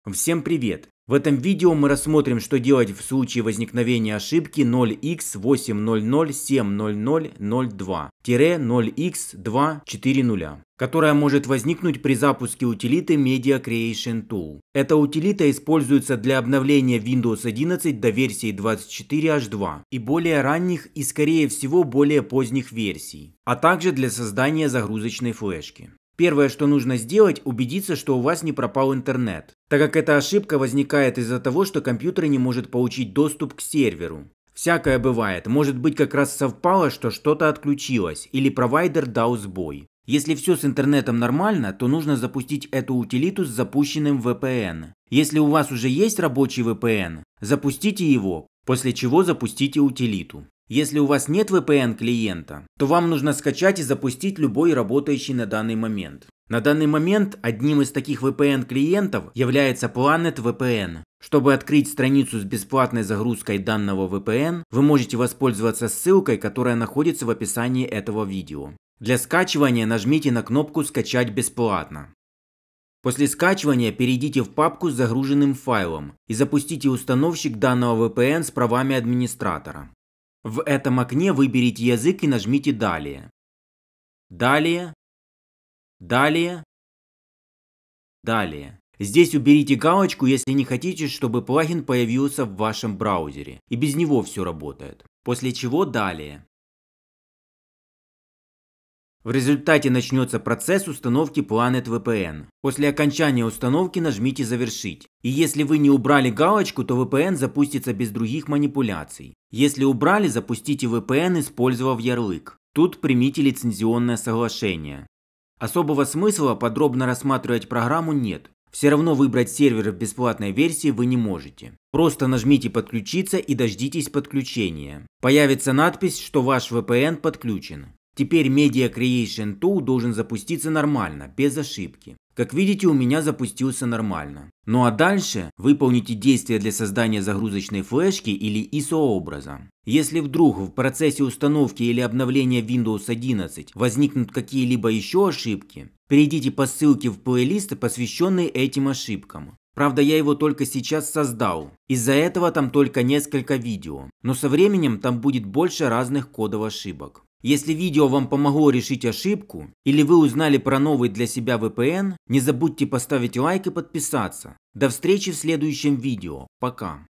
0x80070002 - 0x20000 При запуске Media Creation Tool для установки Windows 11/10
В этом видео я расскажу, как решить ошибку 0x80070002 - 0x20000 при использовании Media Creation Tool. Эта утилита используется для обновления Windows 11 до версии 24H2 и более ранних и скорее всего и более поздних версий, а также для создания загрузочной флешки.

00:00 Краткое содержание видеоролика
00:39 Как устранить ошибк
00:56 Где и как скачать VPN
02:08 Проверка работоспособности Media Creation Tool

Плейлист с кодами ошибок: